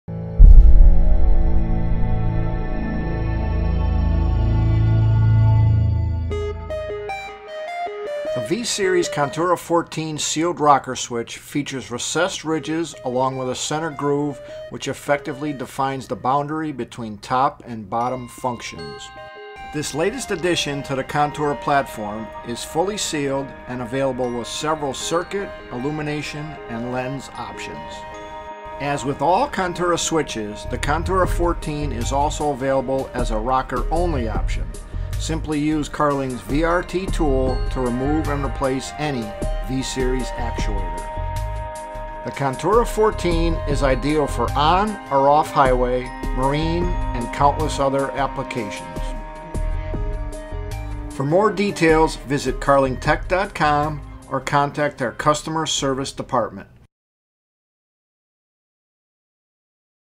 V-Series Contura XIV Rocker Switch
Features a sleek design with an intuitive feel provided by recessed ridges along with a center groove, which effectively defines the boundary between top and bottom switch functions.

• IP66/68 Sealing Protection Above Panel
• 20A 12VDC; 15A 24VDC; 15A 150VAC; 10A 250VAC Ratings
• 1-2 Poles
• UL & CSA Approvals
• Available as Full Switch or Actuator Only